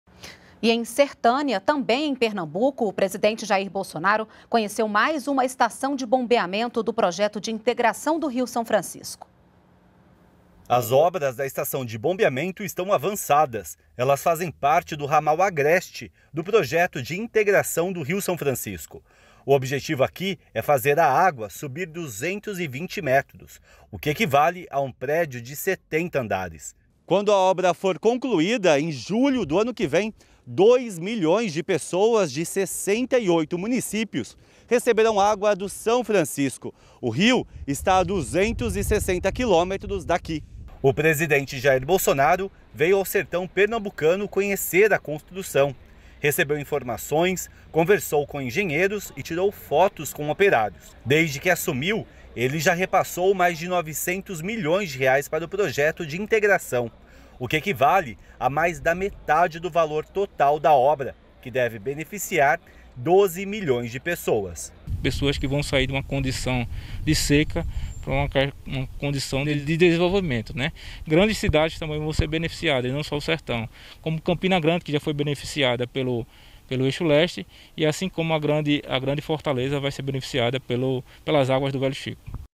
Bolsonaro visita estação de bombeamento de água em Sertânia (PE)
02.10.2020 - O presidente Bolsonaro visitou em Sertânia, Pernambuco, uma estação de bombeamento do projeto de Integração do Rio São Francisco. A obra vai levar água para milhões de pessoas no sertão do estado.